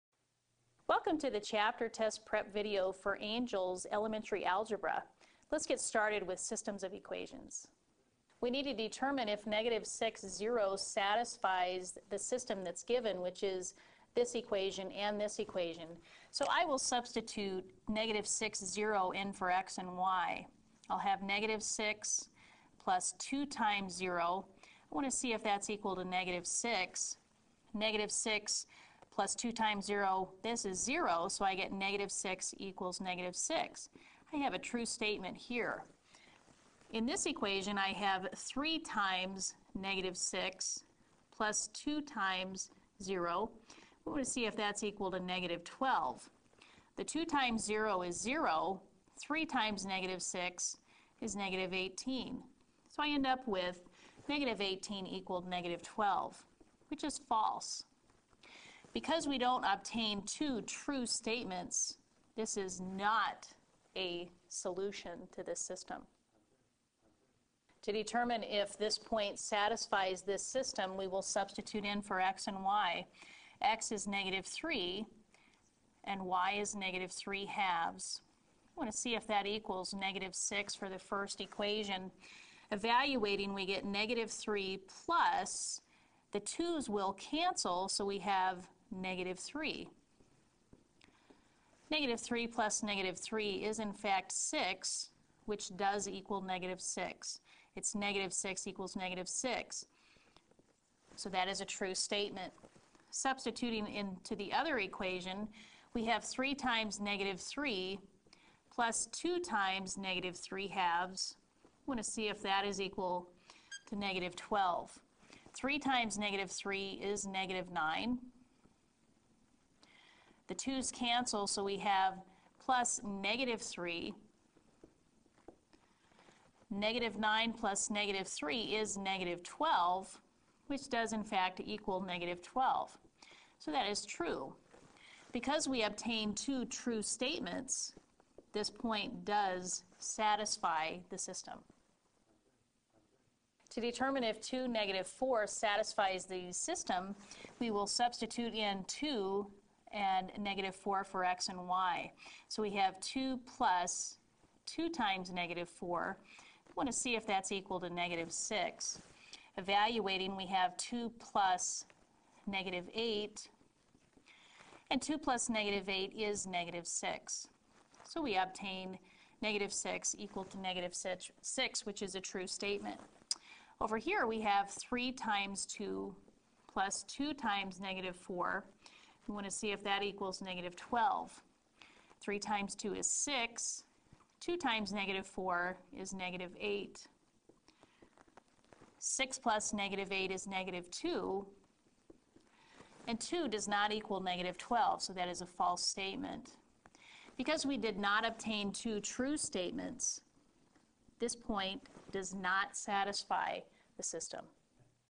Angel Elementary and Intermediate Algebra Ch 9 Ex 1
A math instructor walks you step-by-step through the exercises in the Chapter Tests for Allen Angel and Dennis Runde's Elementary and Intermediate Algebra 4E cr 2011. Video to accompany Chapter 9 of Elementary and Intermediate Algebra 4E cr 2011 by Angel/Runde.